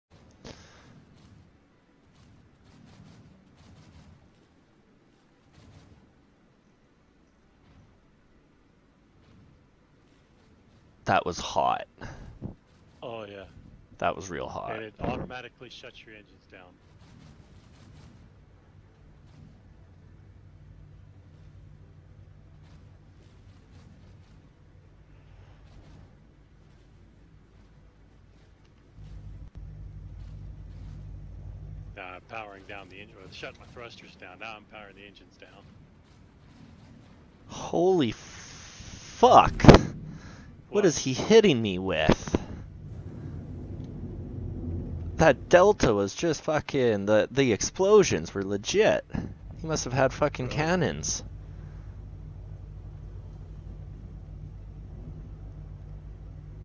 890j Docking while a Mustang tries to interrupt.
Just trying to enjoy the moment when a Mustang Delta with BFG's tries to get my attention.. and well it worked. Though the Station did him in before I could come about.